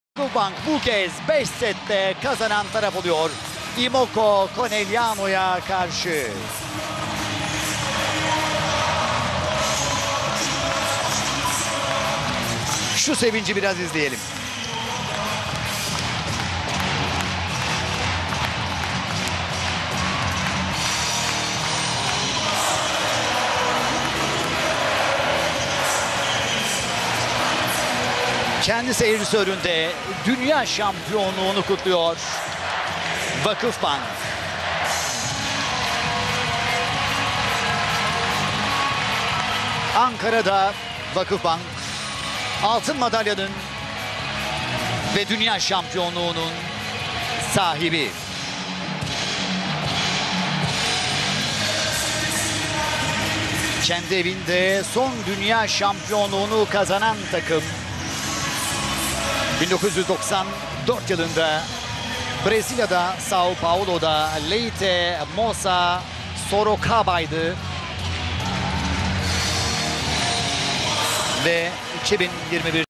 Voleybol VakıfBank Dünya Şampiyonu
FIVB Kadınlar Dünya Kulüpler Şampiyonası'nda 4'üncü kez dünya şampiyonu olarak büyük bir başarıya imza atan VakıfBank Kadın Voleybol Takımımızın oyuncuları başta olmak üzere, teknik ekip ve yönetimini tebrik ediyorum. Elinize, emeğinize sağlık! Sizinle gurur duyuyoruz!